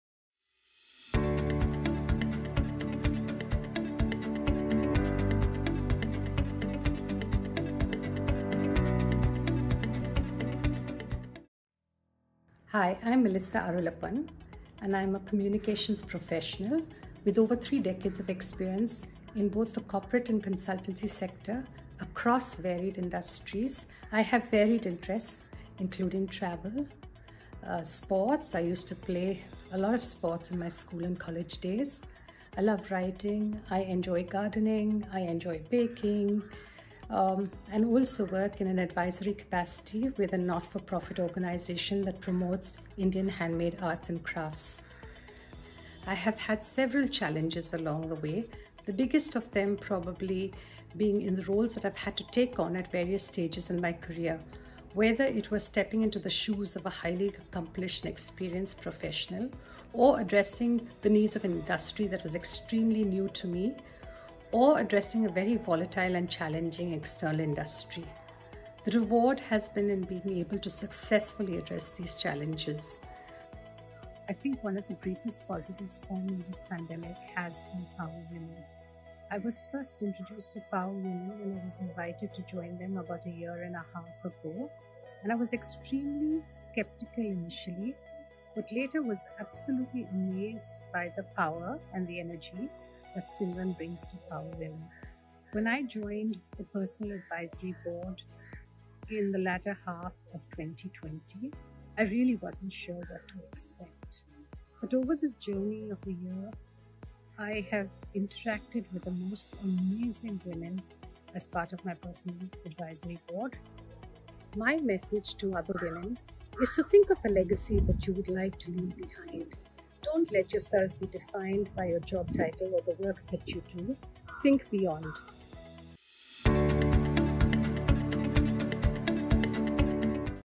Power Story, Melissa Arulappan
Since the day we launched, there have been some incredible FoundingMembers who have brought us this far! One such incredible leader is Melissa Arulappan who has been a real champion of womensupportingwomen. We are so grateful to have Melissa as a FoundingMember, bringing about change and impacting lives of womenleaders.

Melissa is the Head of Corporate Communications at IQVIA and brings with her over three decades of experience in Communications and PublicRelations. She has worked on entry strategies for large multinational organizations, helped launch Indian and multinational brands in the country, successfully worked with organizations through complex and challenging crisis communication assignments, conceptualized creative, award-winning internal and external communication campaigns, built thought leaders in highly competitive environments and driven focused corporate social responsibility programs.

Thank you Melissa for being such an incredible part of this journey!